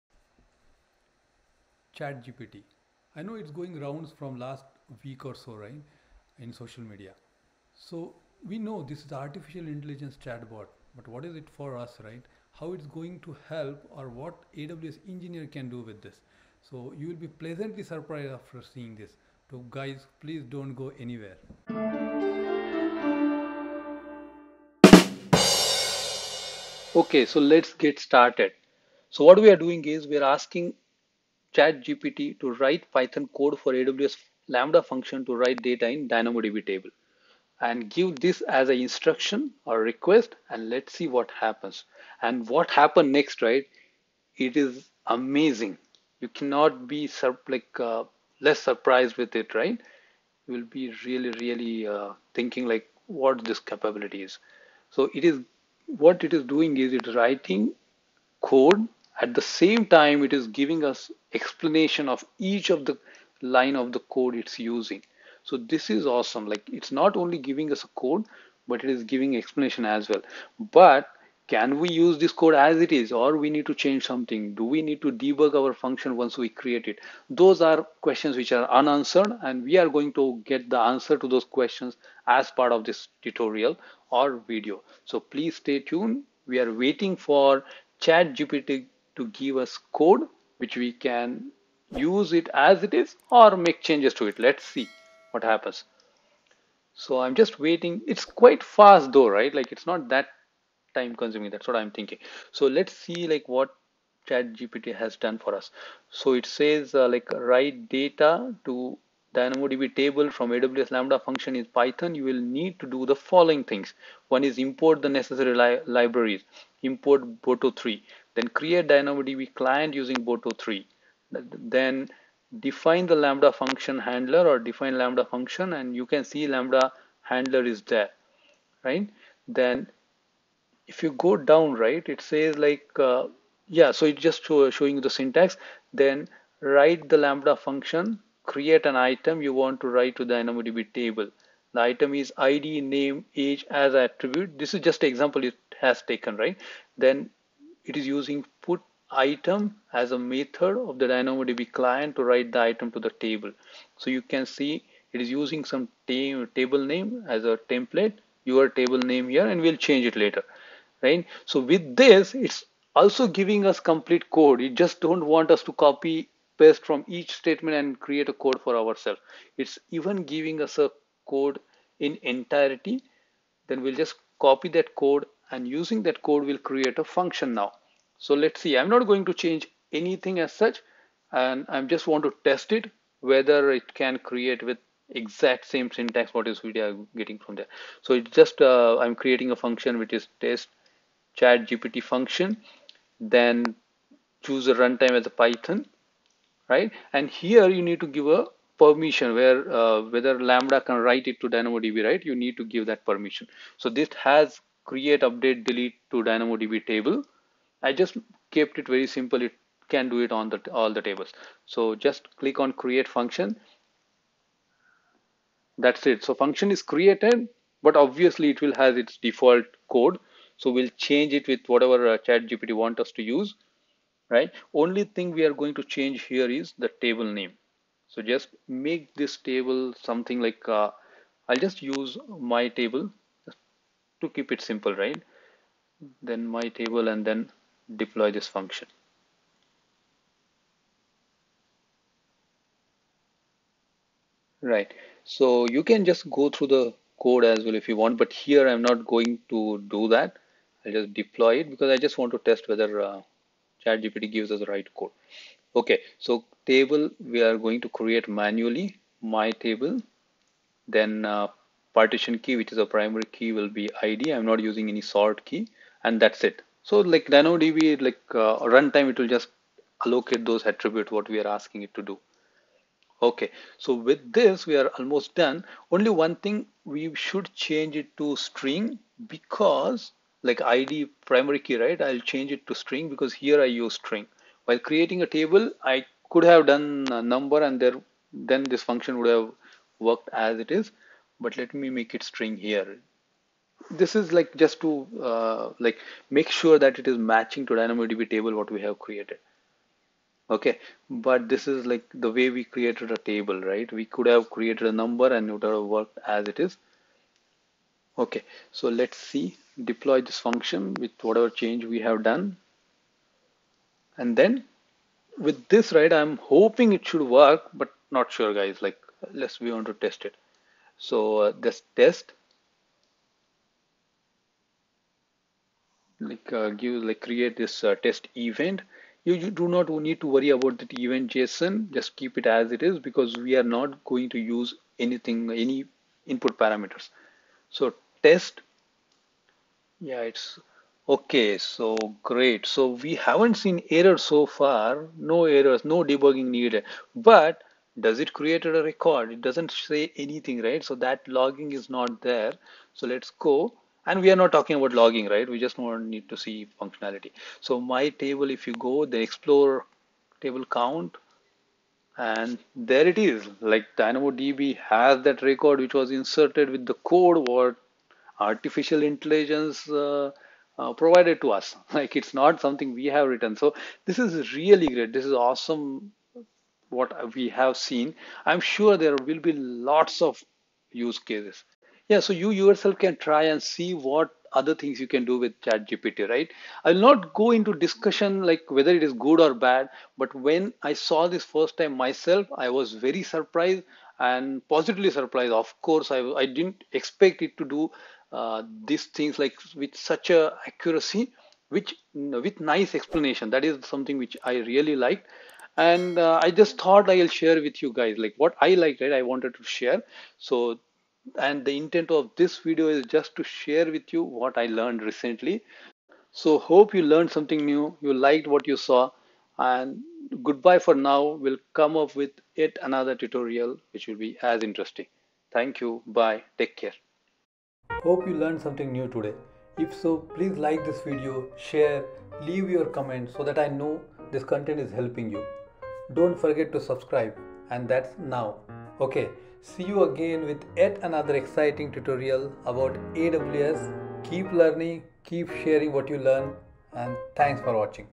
What is ChatGPT and How You Can Use It as AWS Engineer
chatGPT and AWS Engineer working together :-). Imagination knows no bounds. I am amazed at seeing the capabilities of ChatGPT. Here is an attempt to explore this tech with a simple example. I know everyone looks at it differently, and rightly so. I think chatGPT has a potential to enhance productivity; I hope it remains that way.

Learn AWS from the certified solution architect.